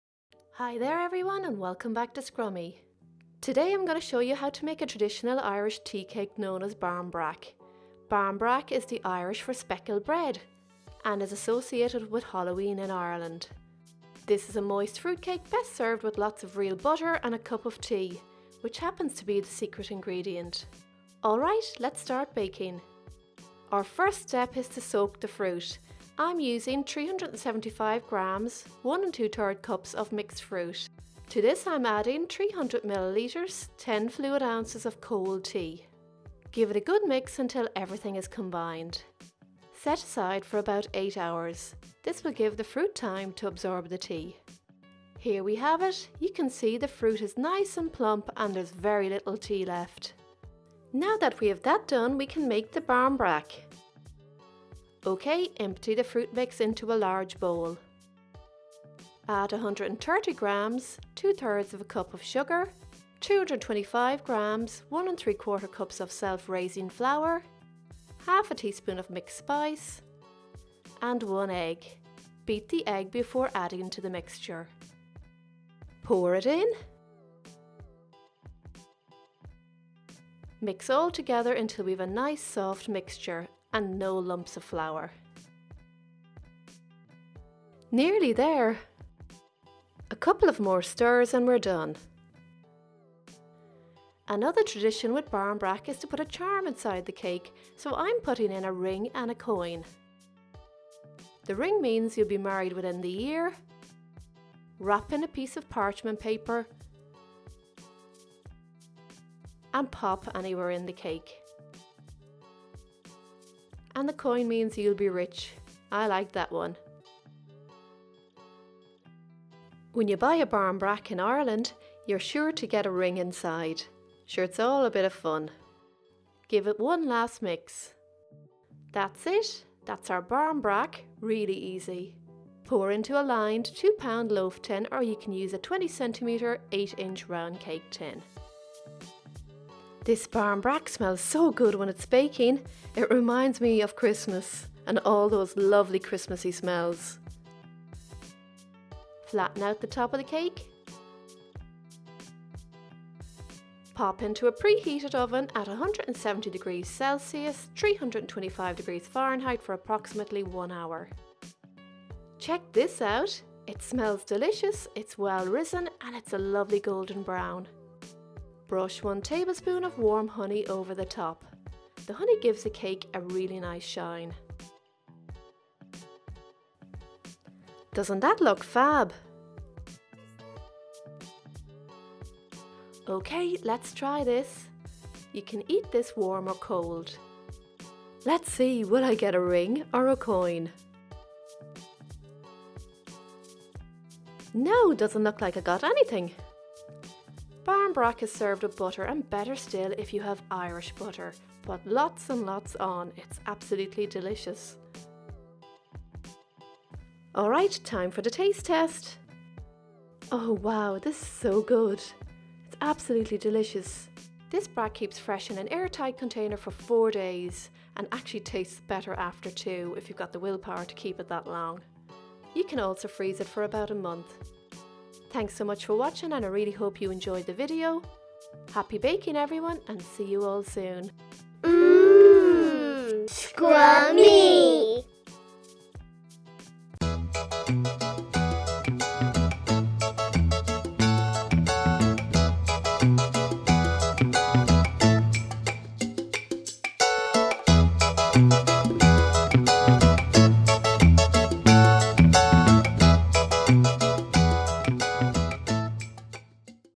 Irish Barmbrack
How to make Irish Barmbrack.

Cook Time 1 hour.
Total Time  1 hour 15 mins.

INGREDIENTS

2  ½  cups (13oz/375g) mixed fruit. (not 1 ⅔ cup as per video)
10 fl oz (300ml) cold black tea.
⅔ cup (4 ½ oz/130g)  caster sugar.
1 ¾ cups (8oz/225g) self-raising flour.
½ teaspoon of mixed spice.
1 egg, beaten.
Ring, coin or other charms.
Honey (for decoration).

METHOD

1. Place the mixed fruit and cold back tea in a bowl to soak for 8 hours.
2. Preheat the oven to 170°C/325°F/Gas Mark 3.
3. Pour the soaked fruit into a large bowl.
4. Add the sugar, self-raising flour, mixed spice and beaten egg.
5. Mix well until all the ingredients are combined.
6. Wrap the ring, coin and any other charms in parchment paper and stir into the mixture.
7. Pour into a lined 2Ib loaf tin or 20cm/8” round cake tin.
8. Bake for approximately 1 hour until well risen and golden brown.
9. Check with a skewer and if the skewer comes out clean then it’s baked.
10. Allow the cake to cool for 1 hour.
11. Brush some melted honey on the top of the loaf.
12. Serve warm or cold with plenty of real butter.
13. Mmm Scrummy!